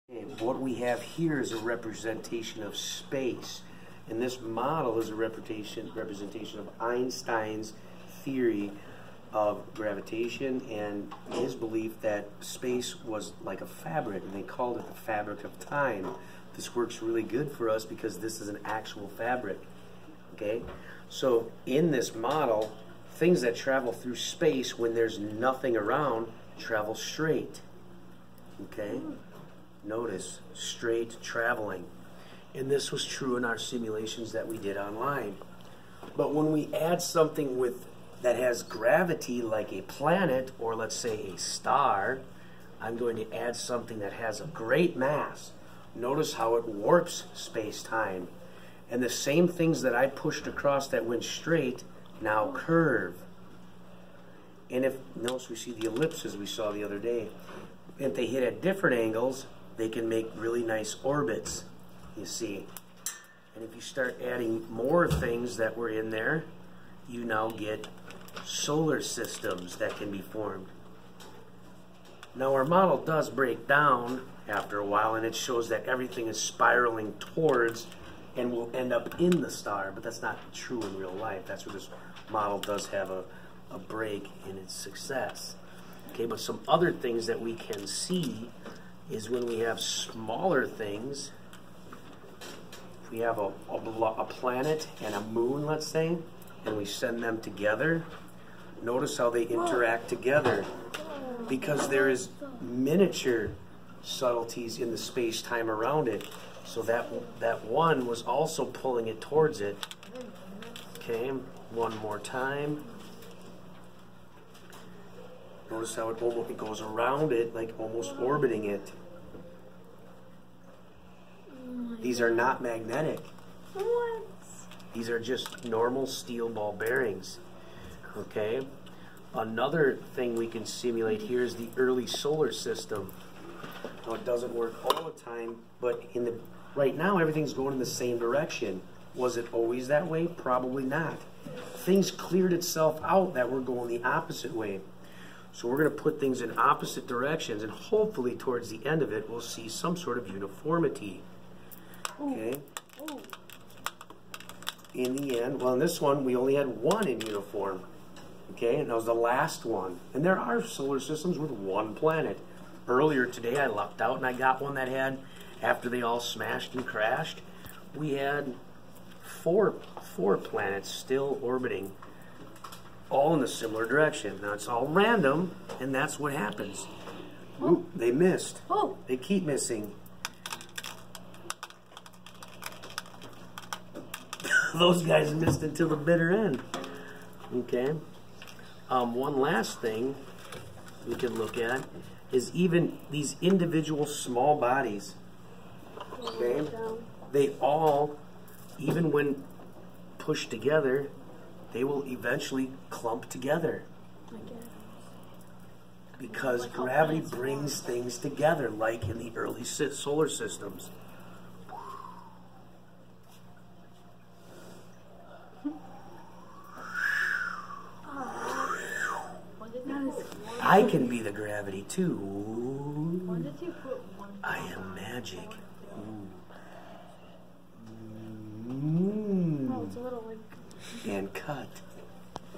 Gravity Simulation - Demonstration of the Fabric of Space
Space and gravity demonstration, gravity wells